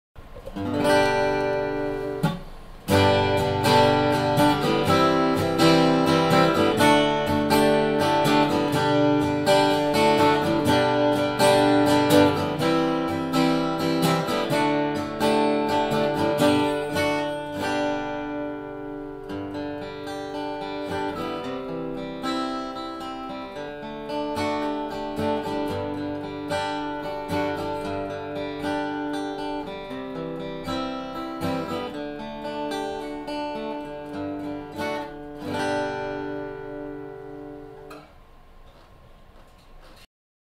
【石橋楽器店】Gibson J-45 VS デモ動画 .MOV
テイアドロップ・ピックガードに、パーフリングが1リングになっているなど、当時の佇まいを彷彿させる仕様になっている、現在のＪ-４５になります。モダン・クラシックからマイナーチェンジされたSTDはバインディングがアンティーク・ホワイトになり、ヘッドのロゴがマザーオブ・パールに変更されています。
ボディトップはシトカ・スプルース、サイド・バックにはマホガニーを使用しています。シトカ・スプルースの輪郭のくっきりとしたアタック音に、マホガニーの柔らかくブリリアンスな倍音が合わさり、ギブソンならではのサウンドを作り上げてくれます。

また、L.R.Baggs製のElement Activeを搭載しているので、ライブでの活躍も大いに期待できます!!
Ｇｉｂｓｏｎ王道Ｊ-４５。その歴史の片鱗に触れてみてはいかがでしょうか。